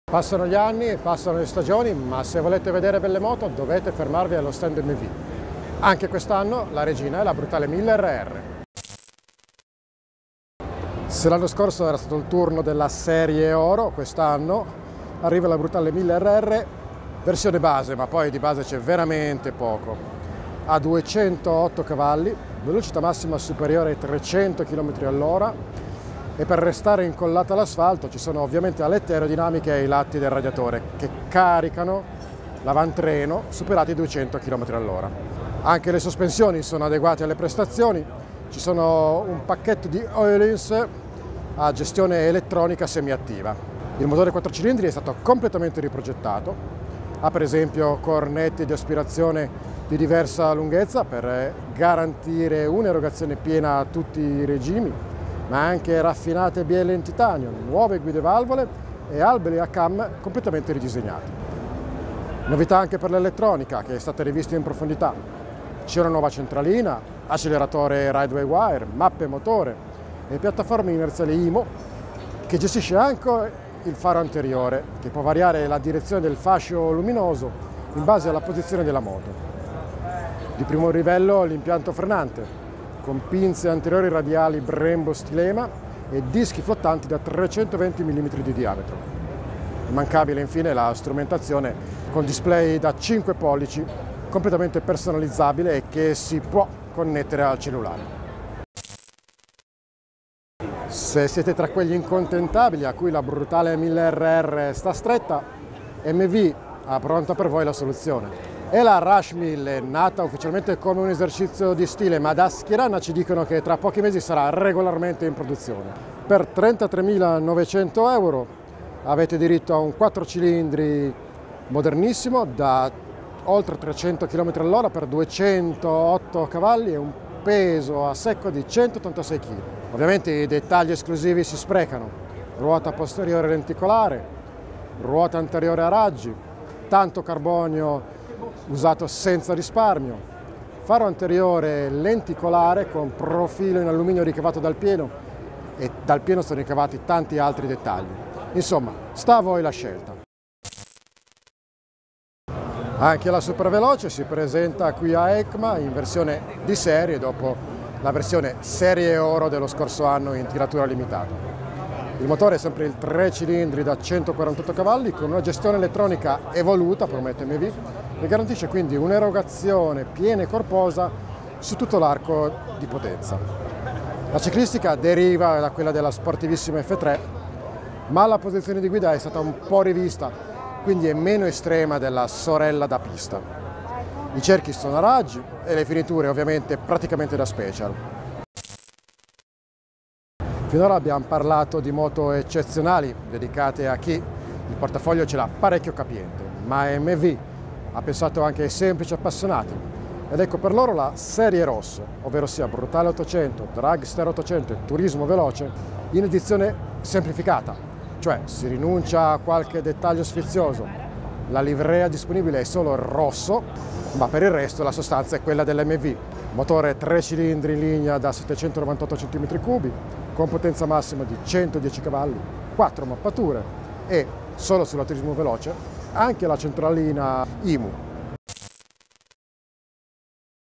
Tutte le novità MV Agusta 2020 presentate a EICMA
Ecco tutte le novità presentate da MV Agusta a EICMA 2019. Scopriamole assieme!

Produzione originale: inSella
Giornalista: Andrea Malossini
Riprese e montaggio: RED